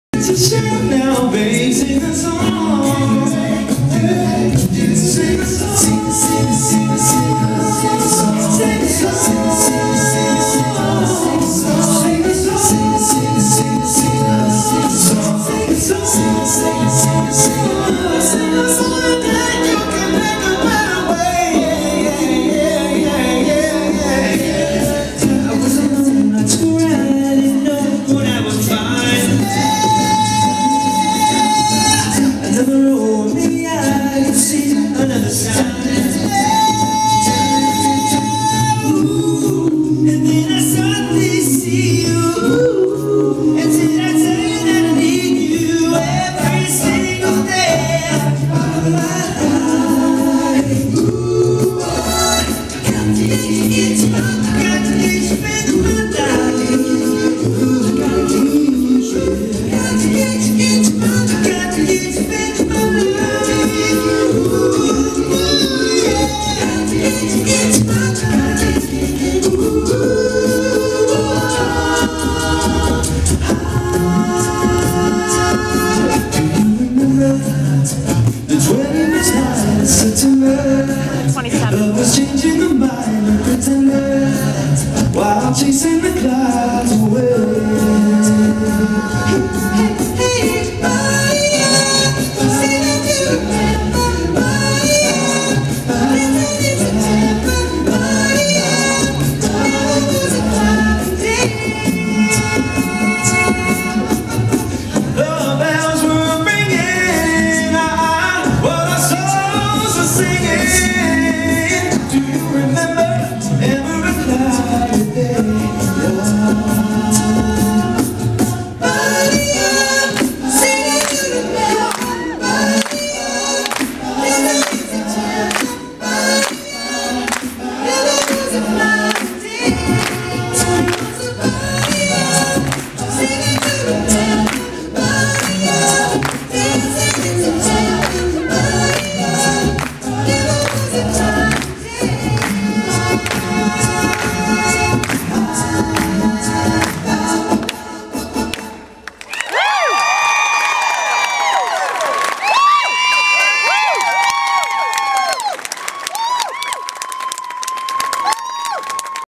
Straight No Chaser- Earth Wind and Fire Medley- ChaserSummit 9-27-13
Chaser Summit in Las Vegas. ChaserSummit sncmusic.com